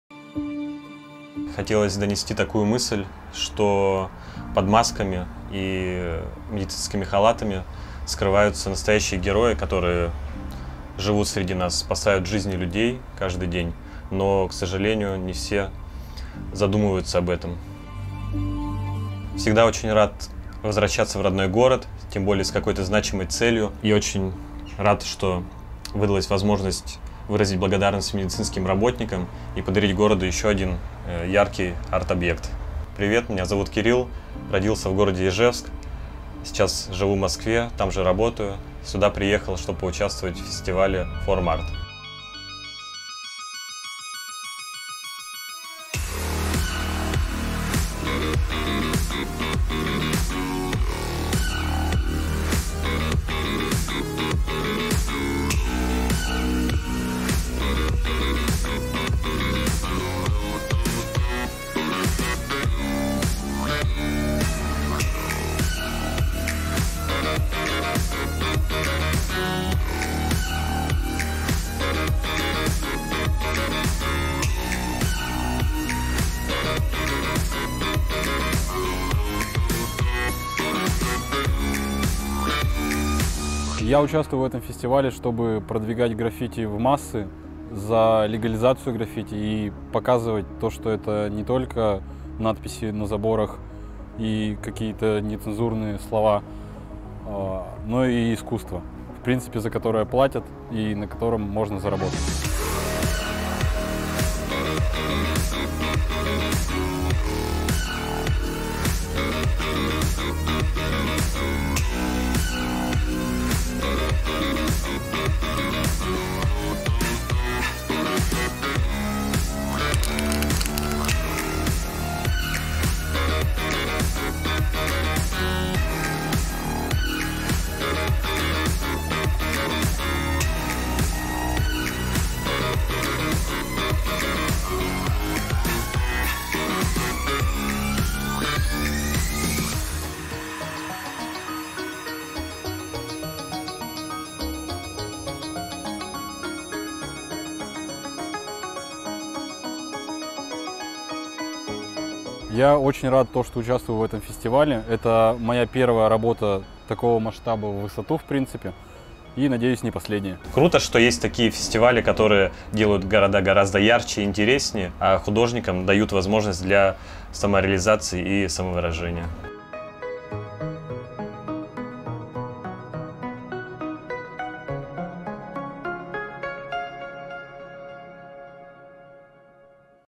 Удмуртская Республика. Доктора-супергерои
Автор: Токарев Кирилл

В глазах большинства людей доктора-это обычные люди, чьих лиц зачастую даже не видно за маской. Но на самом деле многие из них рискуют своей жизнью каждый день ради нас и никто не рассказывает об этих людях. Поэтому все должны знать, что врачи-это настоящие супергерои, и они среди нас.